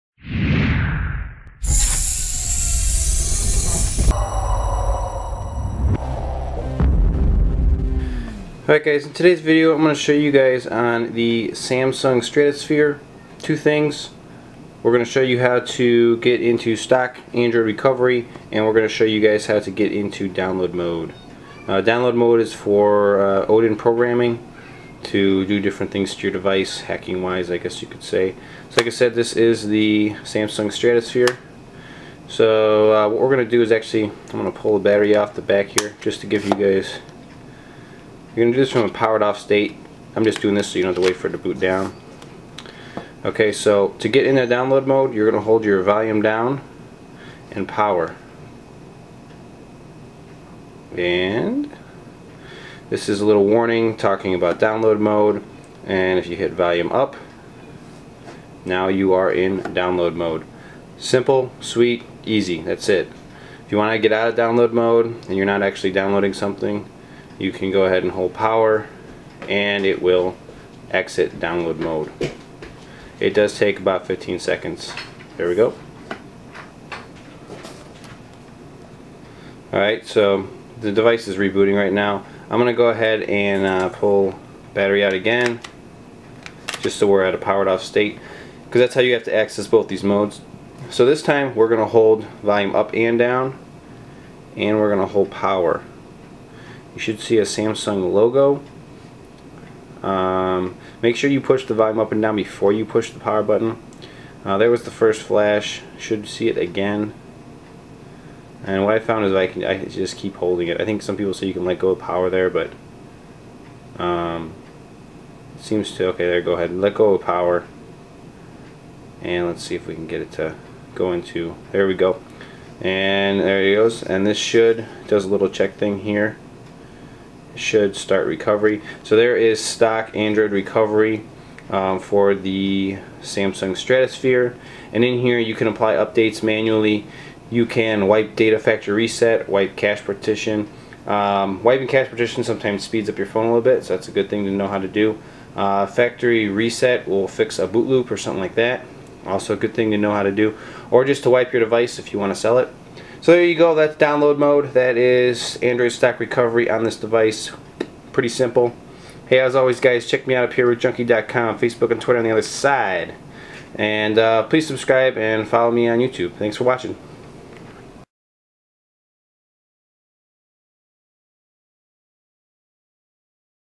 Samsung Stratosphere stock recovery and download mode
How to enter stock recovery and download mode on the Samsung Stratosphere